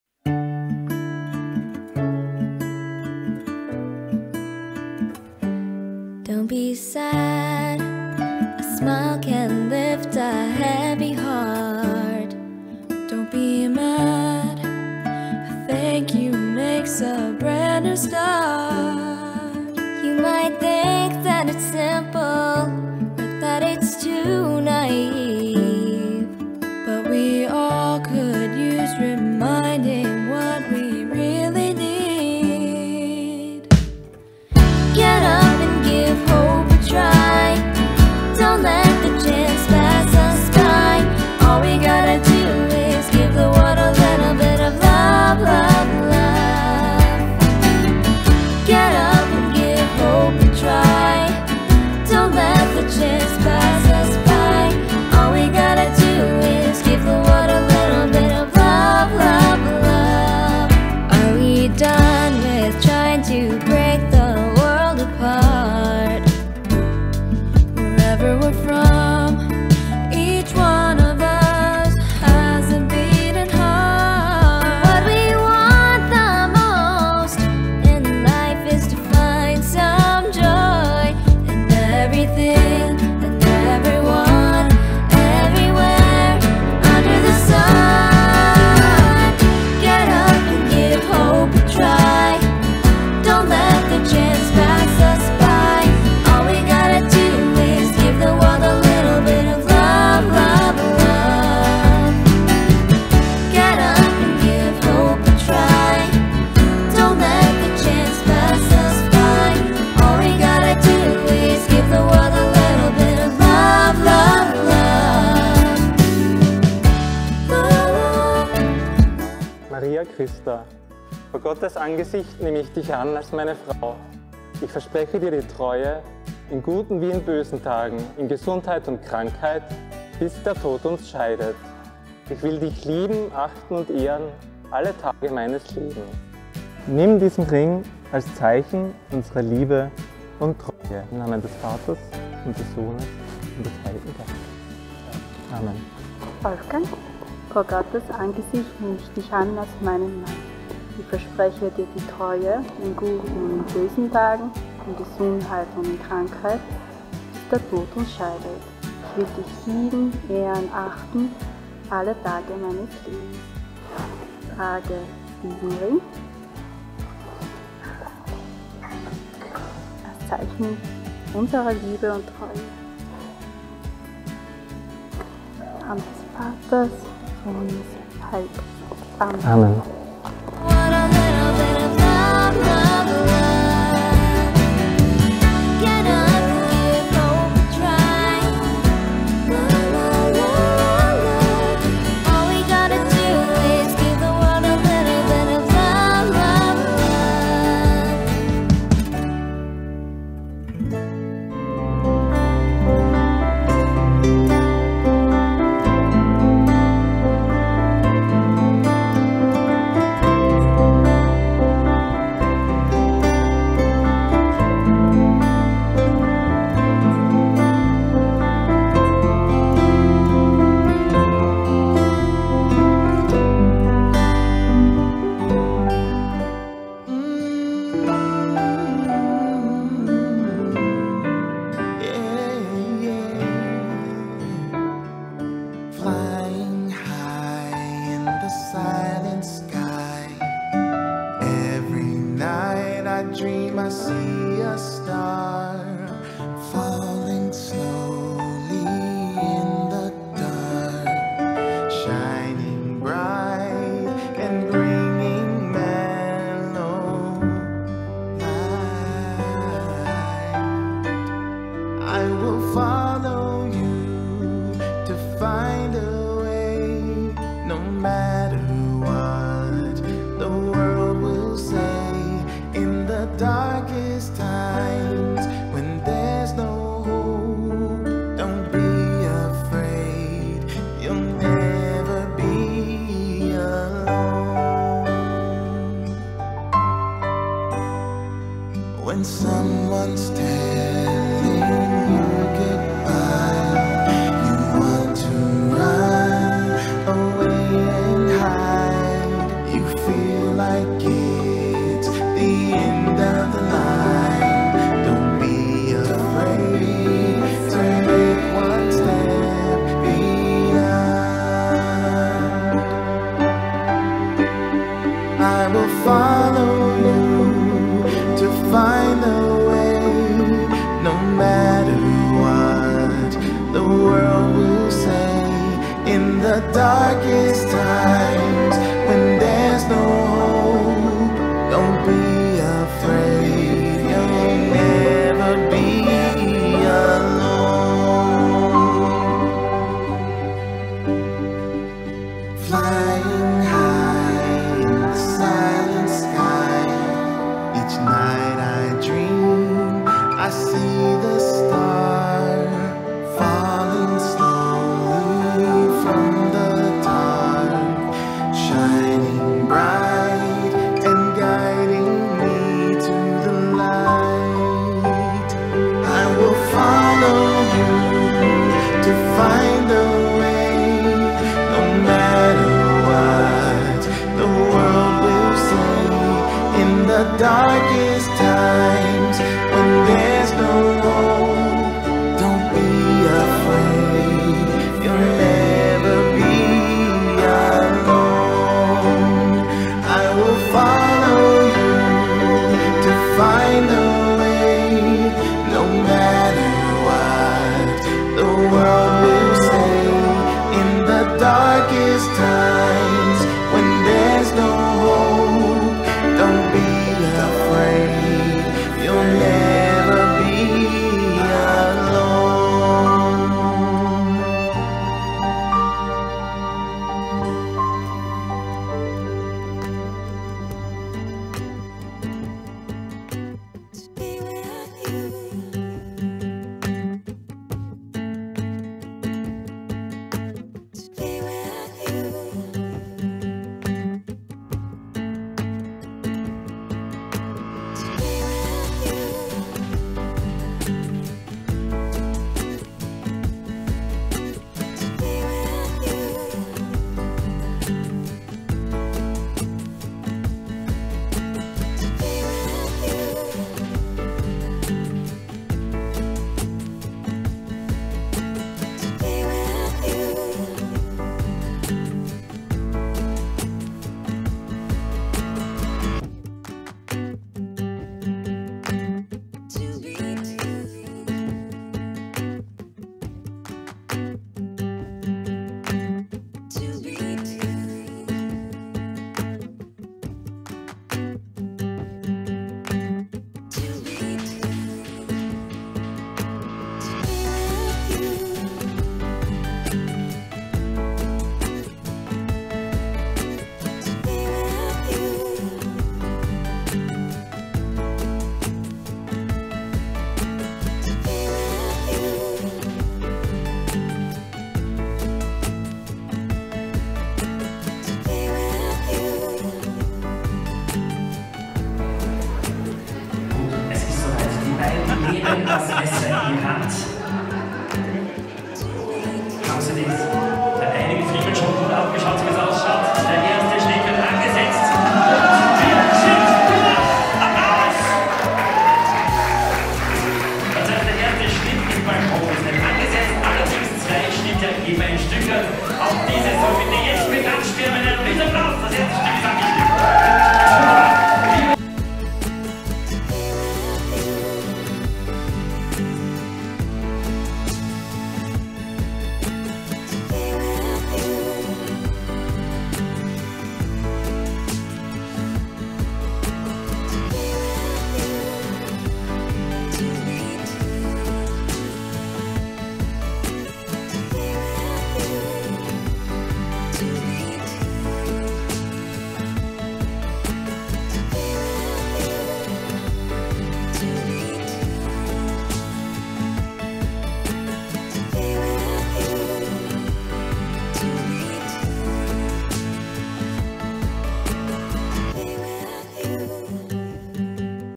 Maria & Wolfgang 09.07.2022
Danke, dass ich euch bei eurem besonderen Tag begleiten durfte.

Ihr seit ein traumhaftes Ehepaar und es hat mir viel Freude gemacht euch zu filmen.

Liebe Grüße Ben
Videograf und Drohnenpilot
für Hochzeiten und mehr